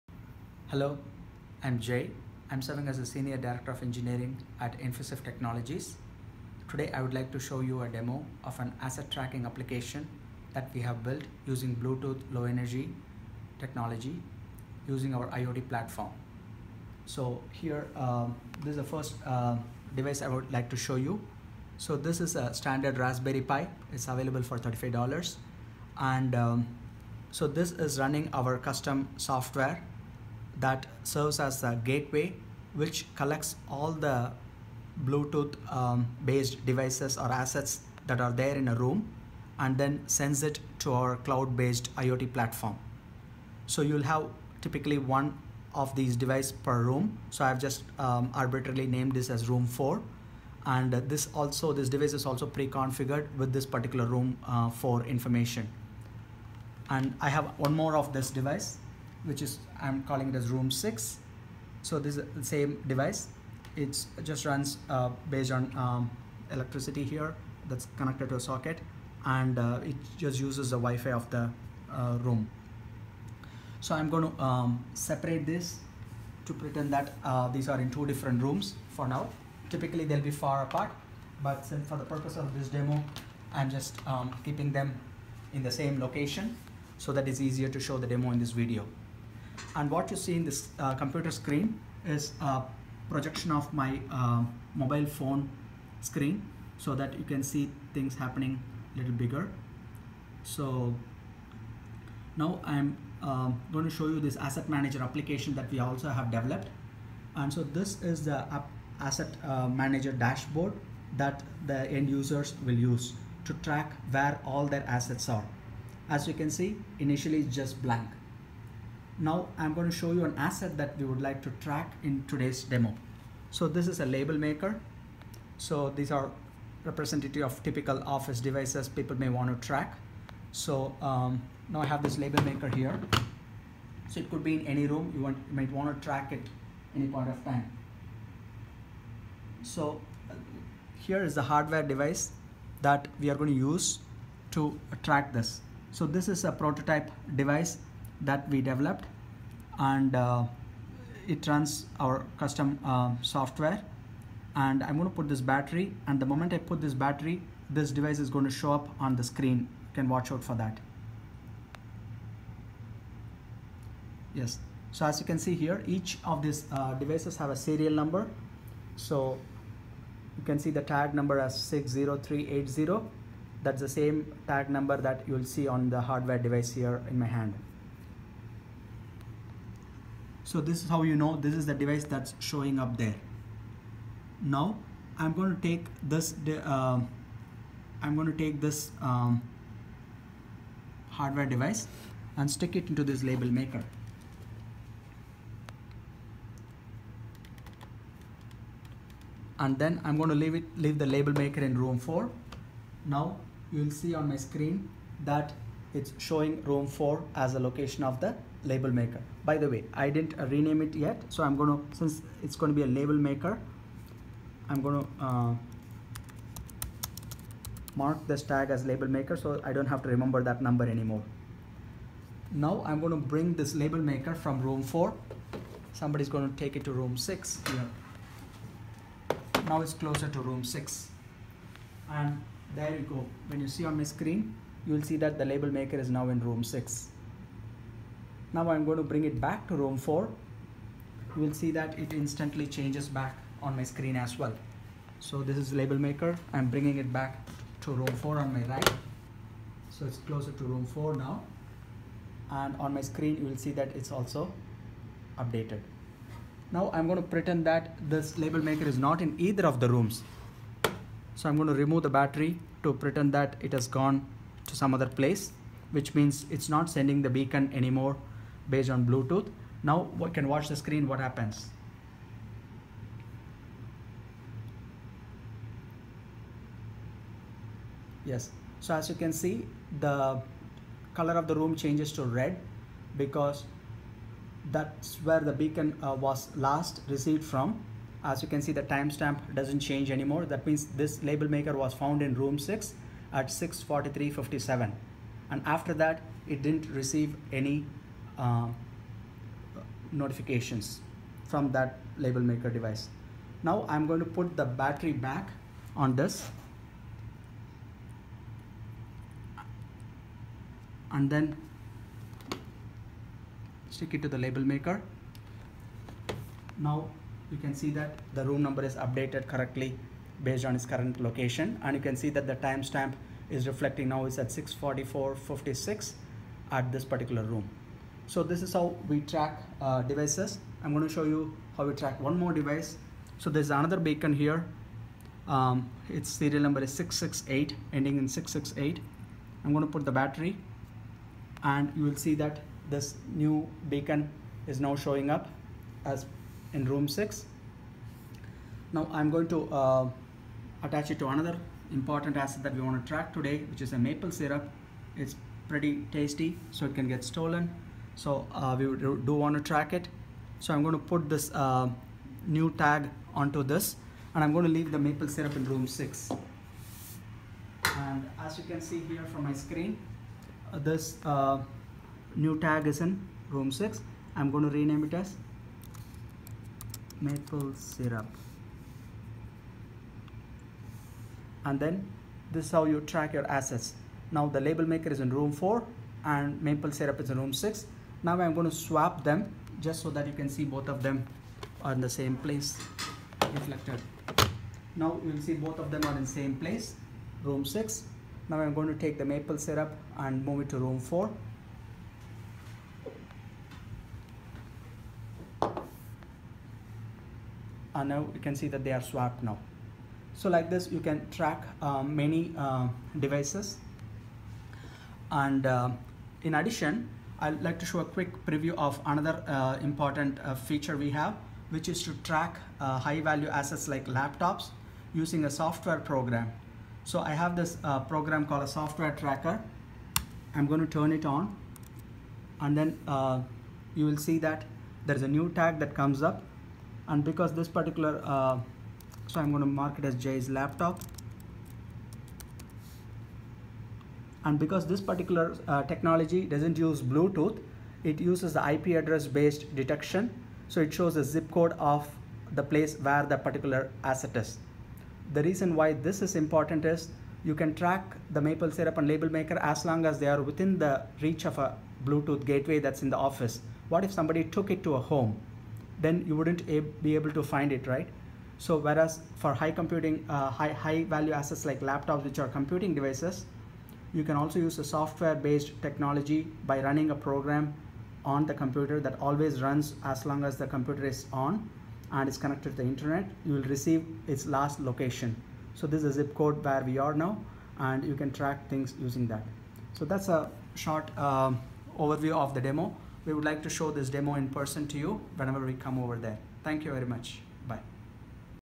BLE-based Asset Tracking Demo
Infiswift Technologies has a full-fledged asset tracking solution based on Bluetooth Low Energy Beacon Technology that communicates to Infiswift's proprietary gateway software that runs on Android Things on a Raspberry Pi. The gateway in turn communicates to Infiswift's IoT Platform in the cloud via MQTT protocol.  The Asset Manager mobile app shows the real-time dashboard status of all assets along with their current locations in green if present or the last known location in red if missing.